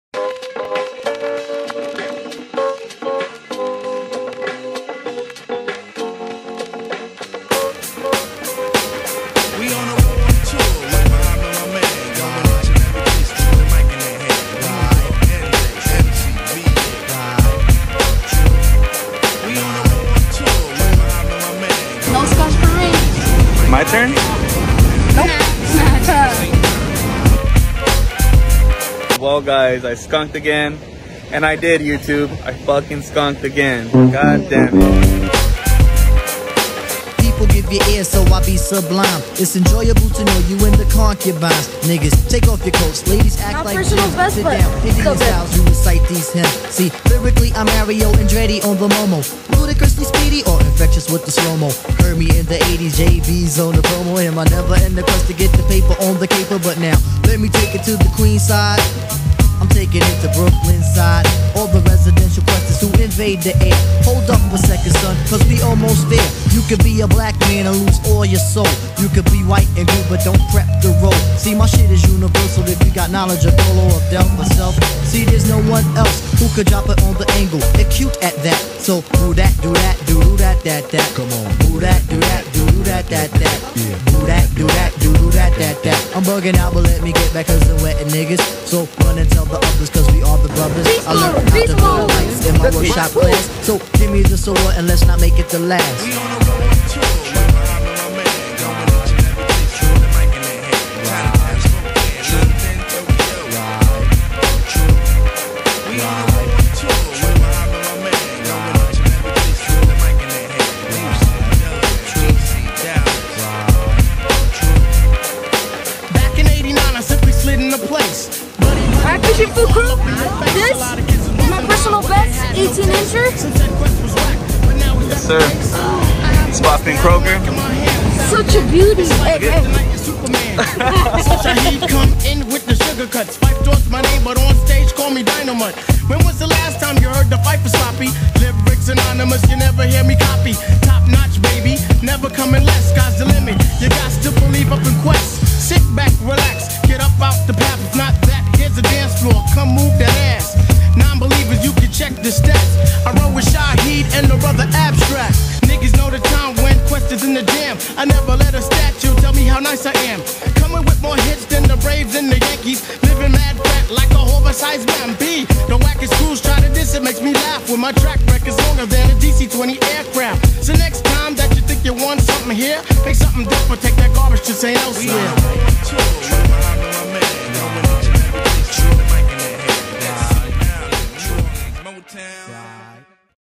🔵 FISHING FOOS CREW @ OCEANSIDE PIER 📍 CATCHING MONSTER SPOT FIN CROAKER 🐟
FISHING FOOS CREW IN OCEANSIDE, CA.📍

MAKE SURE TO WATCH THIS VIDEO IN THE BEST RESOLUTION POSSIBLE IN YOUR VIDEO SETTINGS. (ALL VIDEOS SHOT & EDITED WITH MY IPHONE 12 PRO MAX)